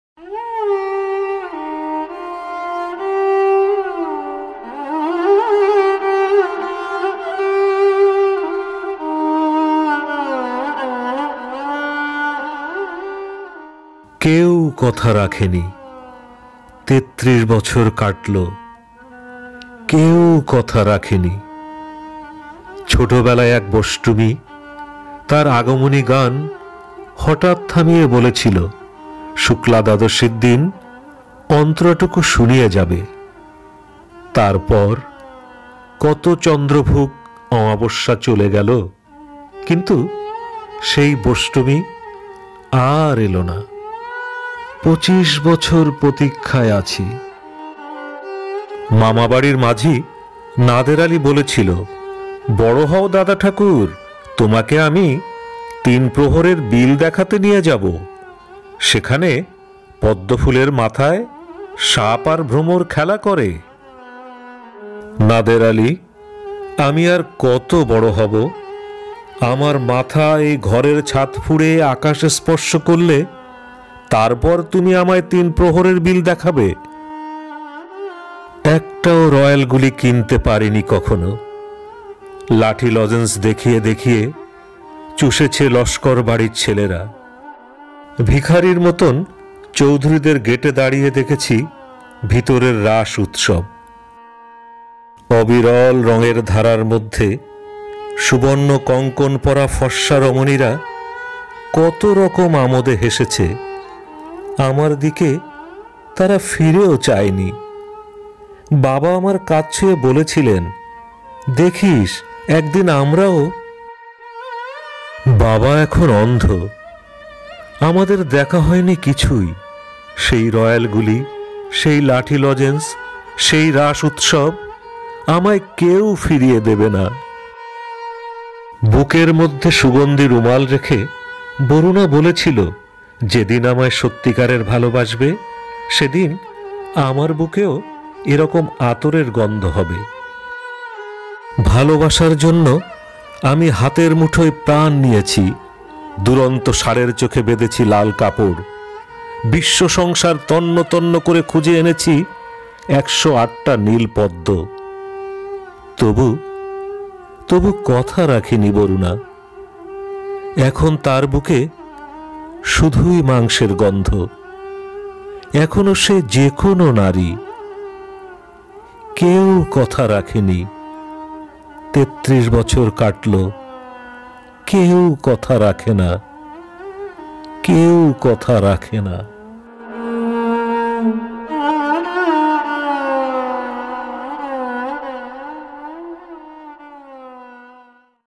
কেউ কথা রাখেনি... আবৃত্তি: আলফাজ হোসেন, কবিতা:সুনীল গঙ্গোপাধ্যায়...Keu kotha rakheni...
কেউ কথা রাখেনি... আবৃত্তি: আলফাজ হোসেন, কবিতা:সুনীল গঙ্গোপাধ্যায়...Keu kotha rakheni...A poem by Sunil Gongopaddhay and Recited by Alfaz Hossain